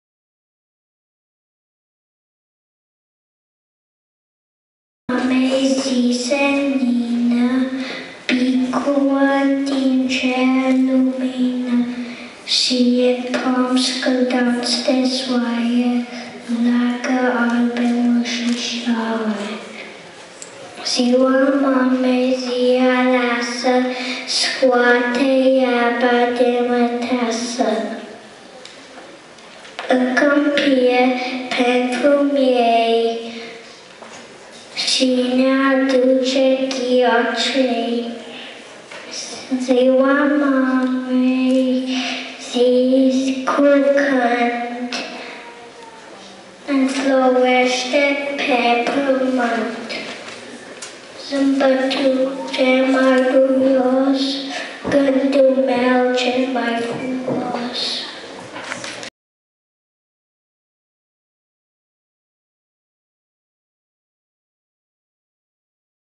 Serbarea de 8 Martie 2015 la Scoala Noastra Oakville, Andrew V
Elevii scolii de limba romana din Oakville, Scoala Noastra, au participat la Serbarea de 8 Martie 2015.
Cei mici au spus poezii, cei din grupa 6-8 ani au pus in scena piesa Ratusca cea Urita, iar cei mari ( 9 – 14 ani) au organizat citeva scenete.

Multumim instructoarelor Mihaela Nastase, Gabriela Covaci, Oana Dragalin, Adriana Cupsa, Monica Tudor pentru perseverenta si dedicatia lor in lucrul cu copiii romani din zona Halton-Hamilton-Peel si celor 97 de copii inscrisi in program.